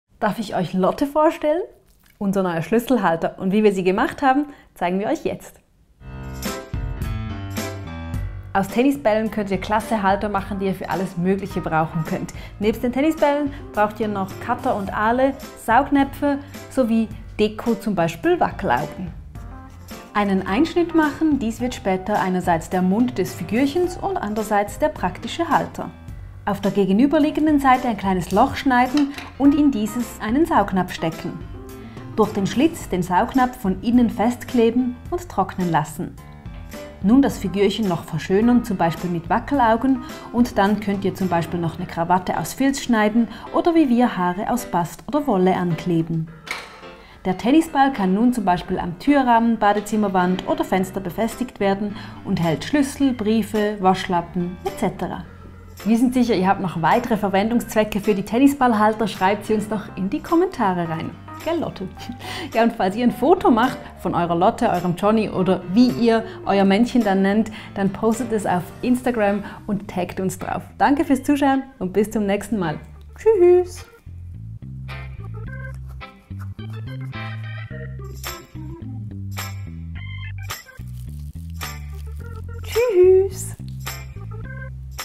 Tennisball Halter für Schlüssel, Briefe, Waschlappen usw.
Tennisbälle sind wunderbar multifunktional, denn sie eignen sich nicht nur für den Sport, zum Spielen - sondern dienen eben auch im Haushalt. Unsere "Lotte" hält zum Beispiel Schlüssel, Waschlappen oder Briefe.
Neue Tipps und Tricks, die das Leben vereinfachen, verschönern oder erleichtern findet Ihr jeden Montag und Freitag um 18h auf unserem Youtube-Kanal.